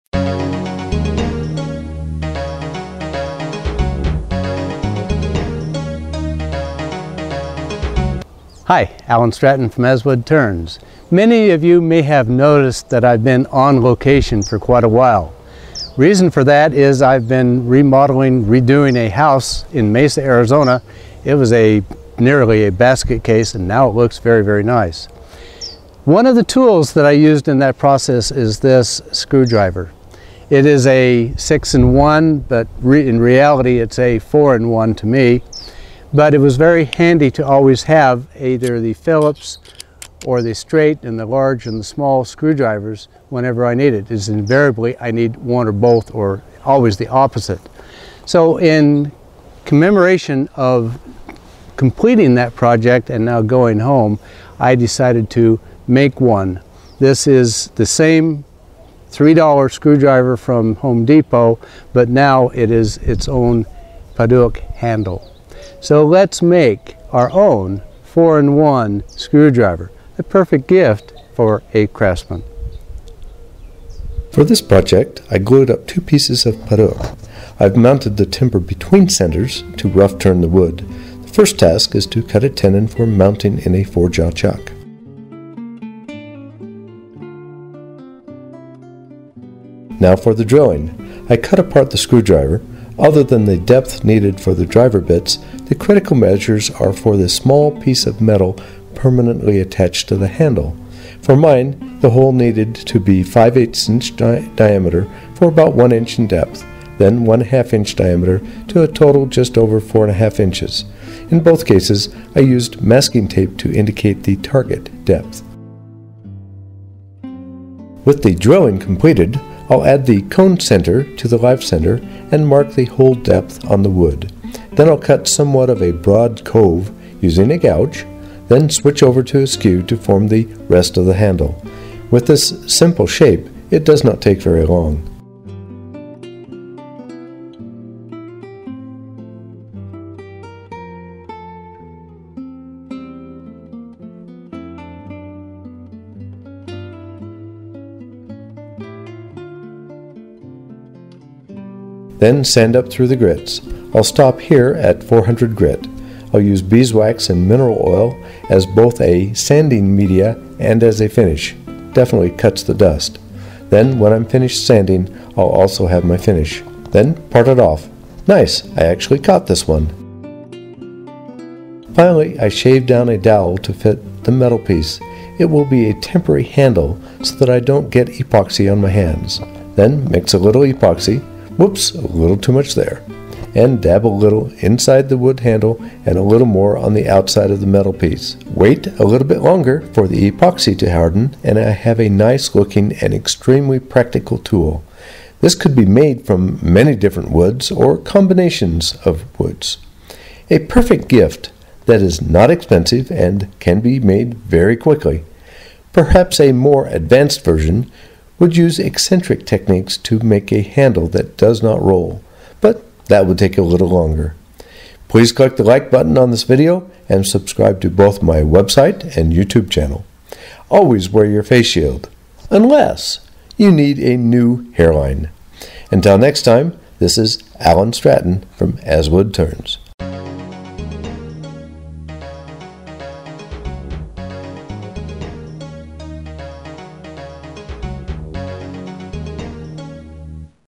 Woodturning A Multi Tip 4 In 1 Screwdriver
I've been on location for remodeling a house in Mesa Arizona. One of the tools I found very useful was an ordinary 4 in 1 screwdriver.
In this project, I dress up that screwdriver. It is a very quick, simple project using only a cheap screwdriver from Home Depot. The handle is Padauk finished with beeswax and mineral oil.